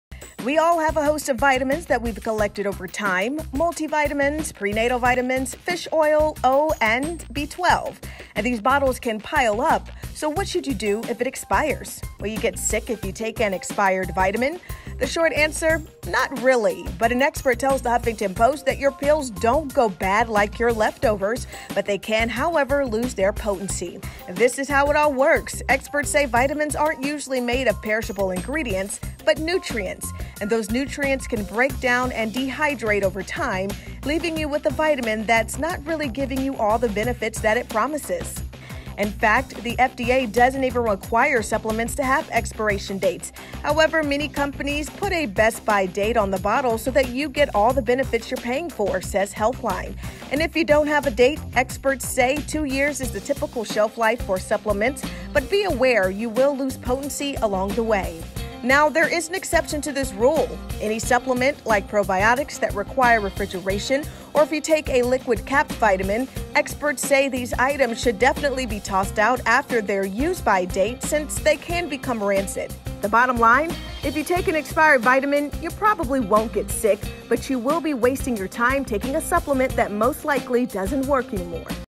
Should You Still Take Vitamins After The Expiration Date?
Will you get sick if you take an expired vitamin? Veuer's Lenneia Batiste has what experts say.